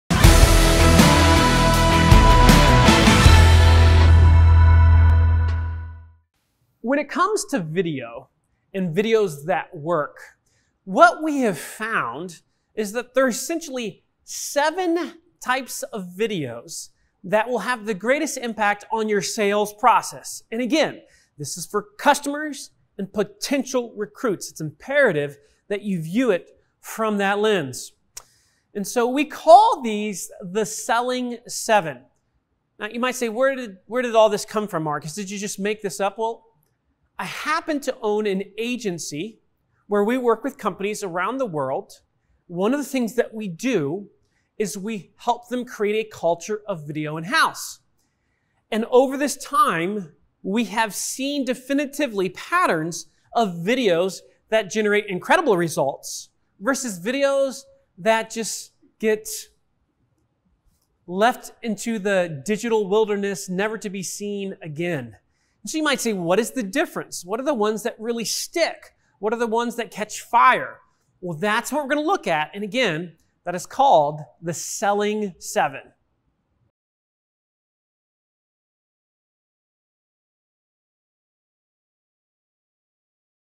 The Selling 7 (Videos That Have the Biggest Impact) with Marcus Sheridan
Marcus goes over the videos that have the biggest impact for your business.